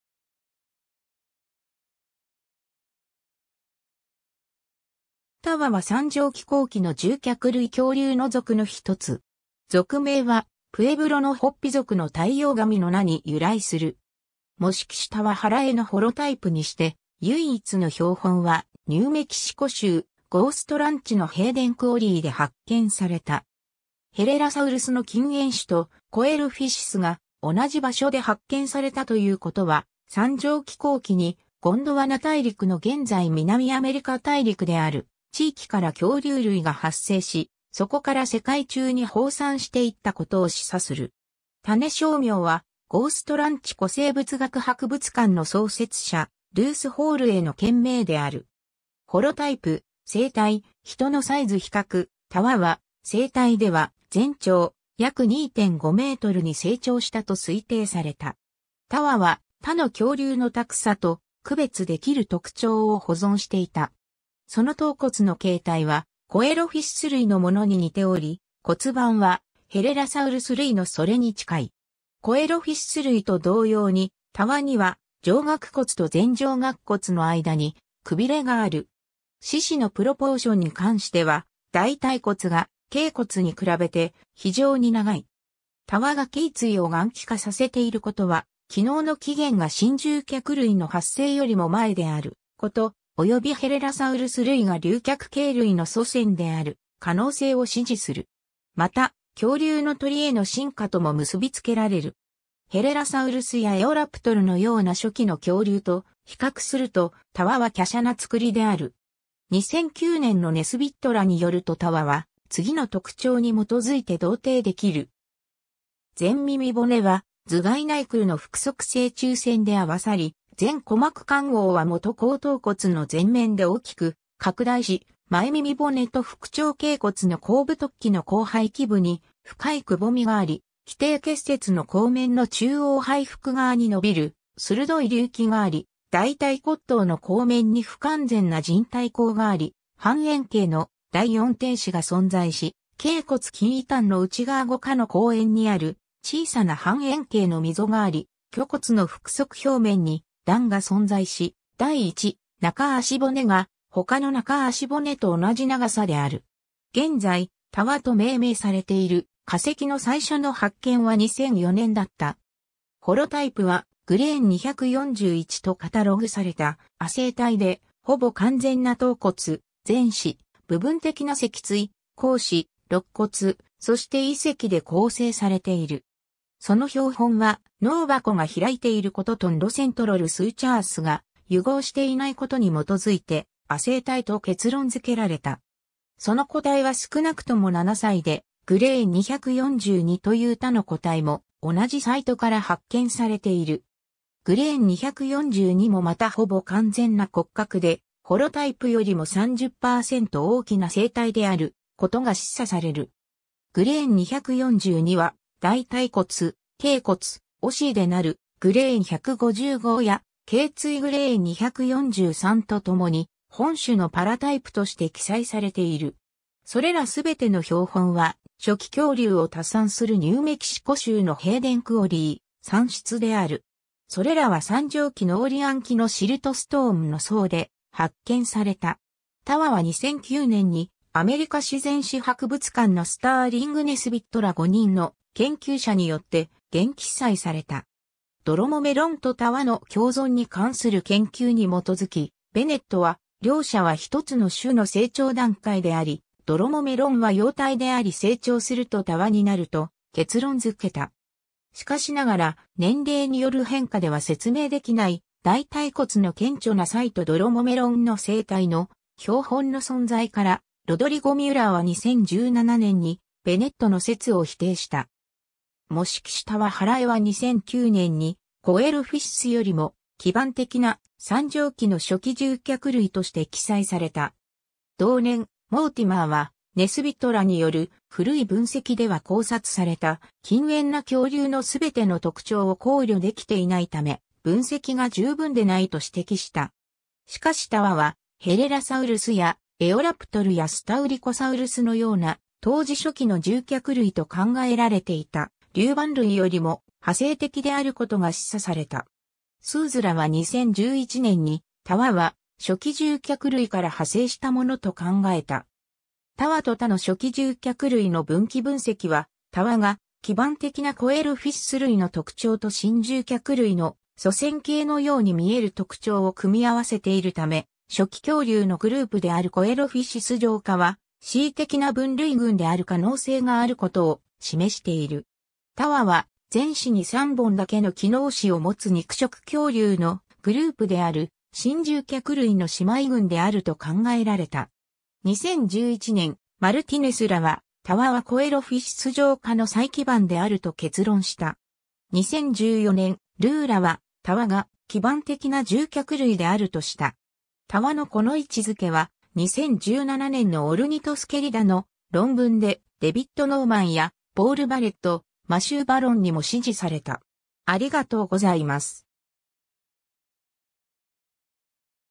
タワ
タワ (Tawa)は三畳紀後期の獣脚類恐竜の属の一つ。
属名はプエブロのホピ族の太陽神の名に由来する。
模式種タワ・ハラエ (Tawa hallae)のホロタイプにして唯一の標本は、ニューメキシコ州ゴーストランチのヘイデン・クオリーで発見された。
ヘレラサウルスの近縁種とコエロフィシスが同じ場所で発見されたという事は、三畳紀後期にゴンドワナ大陸の現在南アメリカ大陸である地域から恐竜類が発生し、そこから世界中に放散していった事を示唆する。
種小名はゴーストランチ古生物学博物館の創設者ルース・ホール (Ruth Hall) への献名である。
 ホロタイプ、成体、ヒトのサイズ比較 タワは成体では全長約2.5mに成長したと推定された。
タワは他の恐竜のタクサと区別できる特徴を保存していた。
その頭骨の形態はコエロフィシス類のものに似ており、骨盤はヘレラサウルス類のそれに近い。
コエロフィシス類と同様に、タワには上顎骨と前上顎骨の間にくびれがある。
四肢のプロポーションに関しては、大腿骨が脛骨に比べて非常に長い。
タワが頸椎を含気化させていることは、気嚢の起源が新獣脚類の発生よりも前である事、およびヘレラサウルス類が竜脚形類の祖先である可能性を支持する。
また恐竜の鳥への進化とも結びつけられる。
ヘレラサウルスやエオラプトルのような初期の恐竜と比較すると、タワは華奢なつくりである。
 2009年のネスビットらによるとタワは、次の特徴に基づいて同定できる。
前耳骨は、頭蓋内腔の腹側正中線で合わさり、前鼓膜陥凹は基後頭骨の前面で大きく拡大し、前耳骨と副蝶形骨の後部突起の後背基部に深いくぼみがあり、基底結節の後面の中央背腹側に伸びる鋭い隆起があり、大腿骨頭の後面に不完全な靭帯溝があり、半円形の第四転子が存在し、脛骨近位端の内側後顆の後縁にある小さな半円形の溝があり、距骨の腹側表面に「段」が存在し、第1中足骨が他の中足骨と同じ長さである。
 現在タワと命名されている化石の最初の発見は2004年だった。
ホロタイプはGR 241 とカタログされた亜成体で、ほぼ完全な頭骨、前肢、部分的な脊椎、後肢、肋骨、そして胃石で構成されている。
その標本は脳函が開いている事と neurocentral sutures(神経弓と椎体の間の縫合線)が癒合していない事に基づいて亜成体と結論づけられた。
その個体は少なくとも7歳で、GR 242 という他の個体も同じサイトから発見されている。
 GR 242 もまたほぼ完全な骨格で、ホロタイプよりも30%大きな成体であることが示唆される。
GR 242 は、大腿骨、脛骨、尾椎で成る GR 155 や頸椎 GR 243 とともに本種のパラタイプとして記載されている。
 それら全ての標本は、初期恐竜を多産するニューメキシコ州のヘイデン・クオリー産出である。
それらは三畳紀ノーリアン期(約2億1500マン〜2億1300万年前)のシルトストーンの層で発見された。
タワは2009年にアメリカ自然史博物館のスターリング・ネスビット (Sterling Nesbitt)ら五人の研究者によって原記載された。
 ドロモメロンとタワの共存に関する研究に基づき、ベネット (S. Christopher Bennett)は、両者は一つの種の成長段階であり、ドロモメロンは幼体であり成長するとタワになると結論づけた。
しかしながら、年齢による変化では説明できない大腿骨の顕著な差異とドロモメロンの成体の標本の存在から、ロドリゴ・ミュラーは2017年にベネットの説を否定した。
 模式種タワ・ハラエ (Tawa hallae)は2009年にコエロフィシスよりも基盤的な、三畳紀の初期獣脚類として記載された。
同年、モーティマーは、ネスビットらによる古い分析では考察された近縁な恐竜(グアイバサウルス、パンファギア、シノサウルス、ドラコヴェナトル、ロフォストロフェウスなど)のすべての特徴を考慮できていないため、分析が十分でないと指摘した。
しかしタワはヘレラサウルスやエオラプトルやスタウリコサウルスのような、当時初期の獣脚類と考えられていた竜盤類よりも派生的であることが示唆された。
 スーズ (Sues)らは2011年に、タワは初期獣脚類から派生したものと考えた。
タワと他の初期獣脚類の分岐分析は、タワが基盤的なコエロフィシス類の特徴と新獣脚類の祖先形のように見える特徴を組み合わせているため、初期恐竜のグループであるコエロフィシス上科は恣意的な分類群である可...